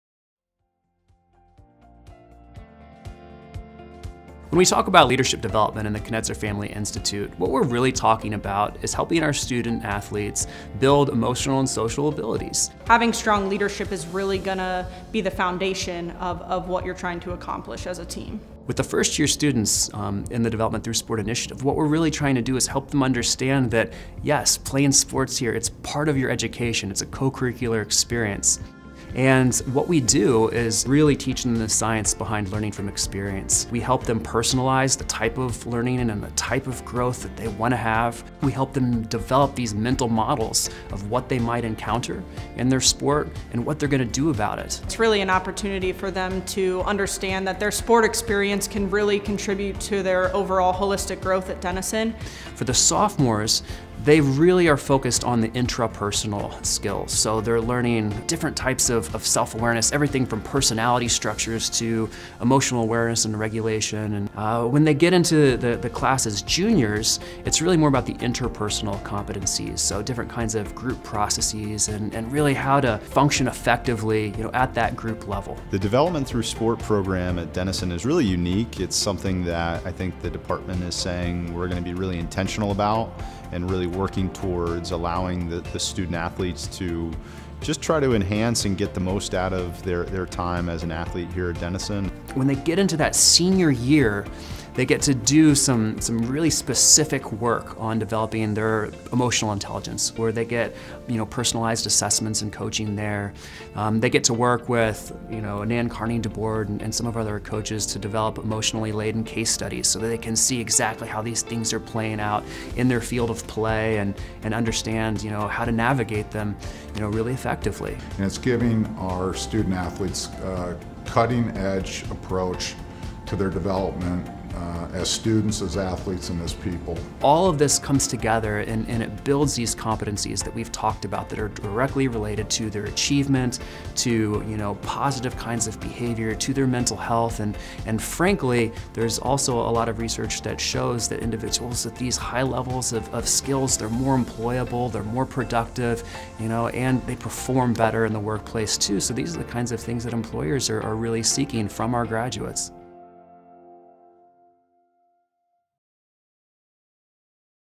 Knetzer Institute on Leadership Development
Building better leaders is one of the fundamental pillars of the Knetzer Family Institute. Find out how we execute that mission by hearing from the coaches and professors who are making it happen on a daily basis.